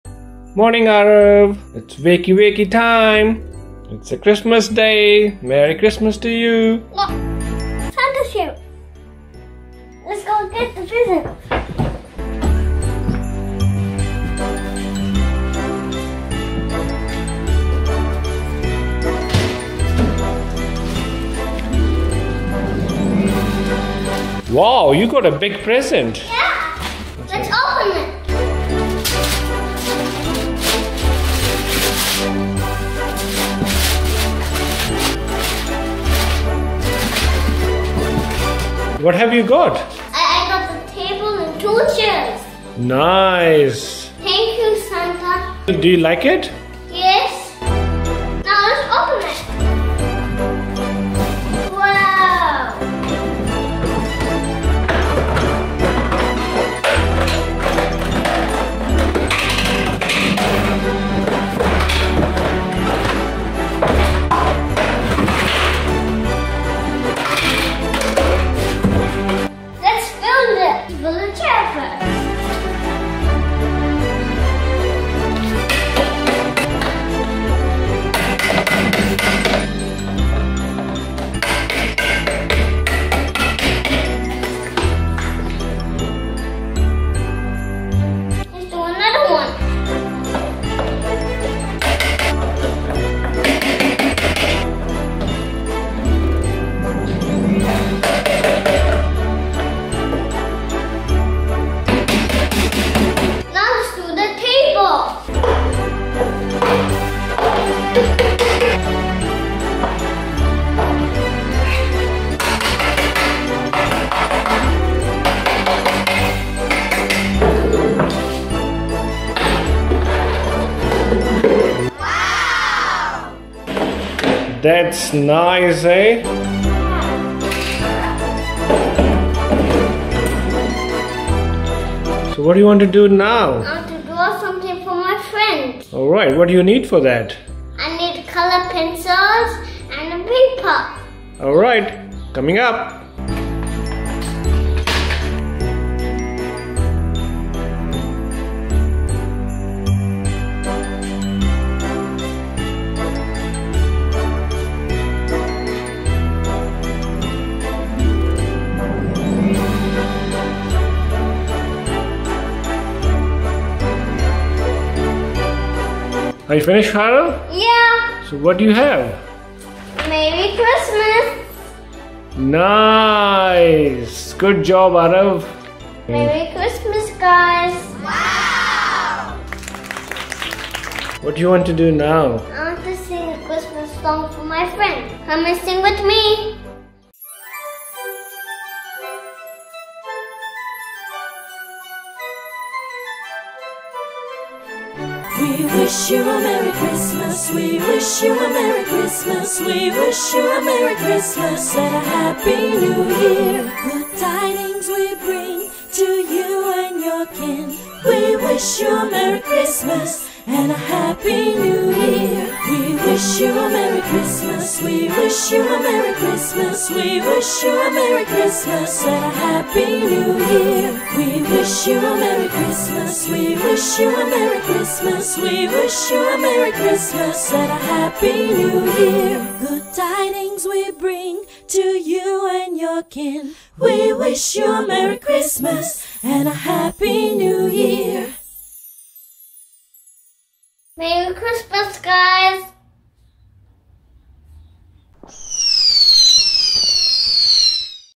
Merry Christmas and Happy New year from Aarav! Kids toys! Fun time for kids! Love Christmas
Aarav wants to wish Merry Christmas and Happy New Year to his family and friends.
We wish all of you a Merry Christmas and Happy New Year. Wish you all Happy and Safe holidays.